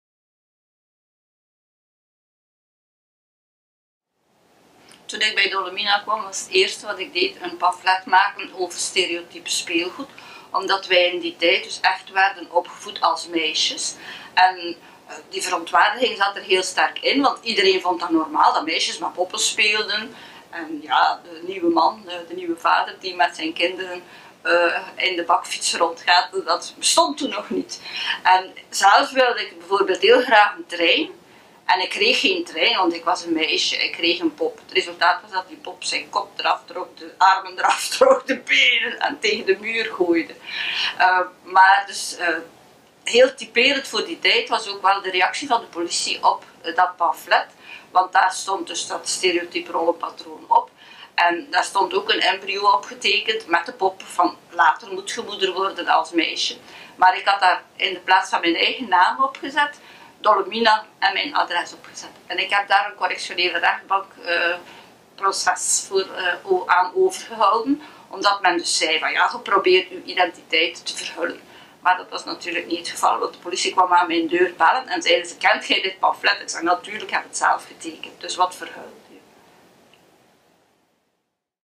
Moniek Darge: "Stereotiep speelgoed"
Moniek Darge maakte één van de eerste pamfletten van Dolle Mina Gent, over stereotiep speelgoed.